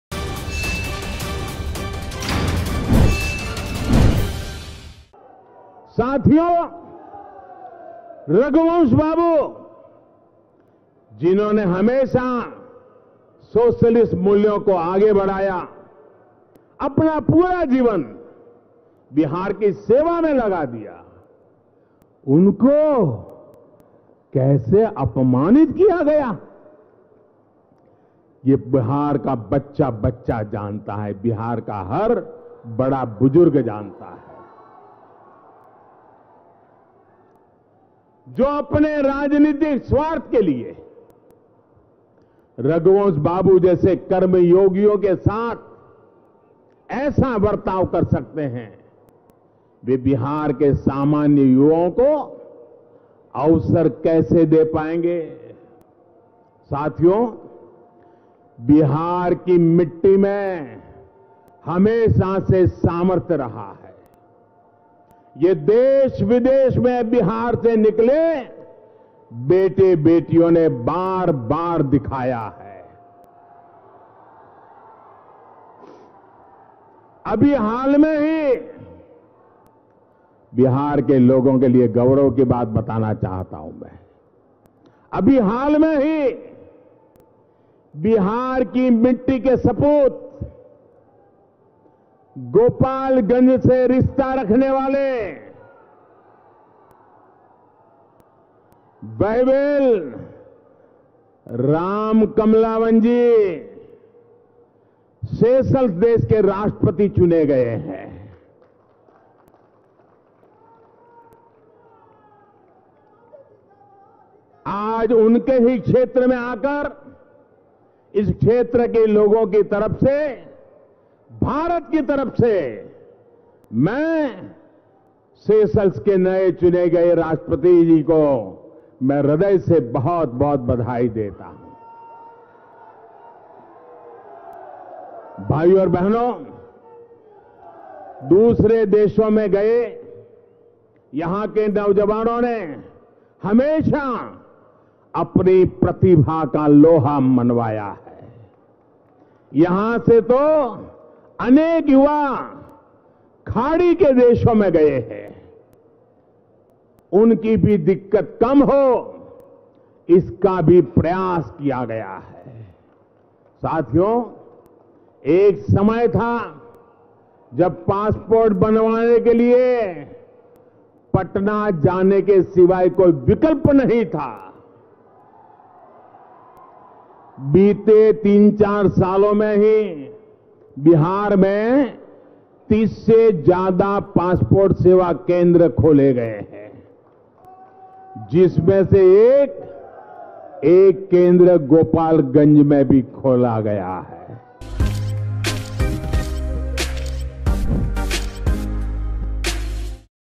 Bihar Election 2020: PM Modi ने Raghuvansh Prasad Singh के अपमान को लेकर क्या कहा? | वनइंडिया हिंदी
Prime Minister Narendra Modi discussed former RJD leader Raghuvansh Prasad Singh. The PM said that there were such leaders who always carried forward the socialist values, and devoted their whole life to the service of Bihar. The whole of Bihar has seen how they were insulted. PM said that for his political selfishness, who can treat such karmayogis like Raghuvansh Babu, how will he be able to give opportunity to ordinary youth of Bihar. Watch video,

प्रधानमंत्री नरेंद्र मोदी ने पूर्व आरजेडी नेता रघुवंश प्रसाद सिंह की चर्चा की. पीएम ने कहा कि वैसे ऐसे नेता थे जिन्होंने हमेशा सोशलिस्ट मूल्यों को आगे बढ़ाया, अपना पूरा जीवन बिहार की सेवा में लगा दिया. उनको कैसे अपमानित किया गया, ये पूरे बिहार ने देखा है. पीएम ने कहा कि जो अपने राजनैतिक स्वार्थ के लिए रघुवंश बाबू जैसे कर्मयोगियों के साथ ऐसा बर्ताव कर सकते हैं, वो बिहार के सामान्य युवाओं को अवसर कैसे दे पाएंगे. देखें वीडियो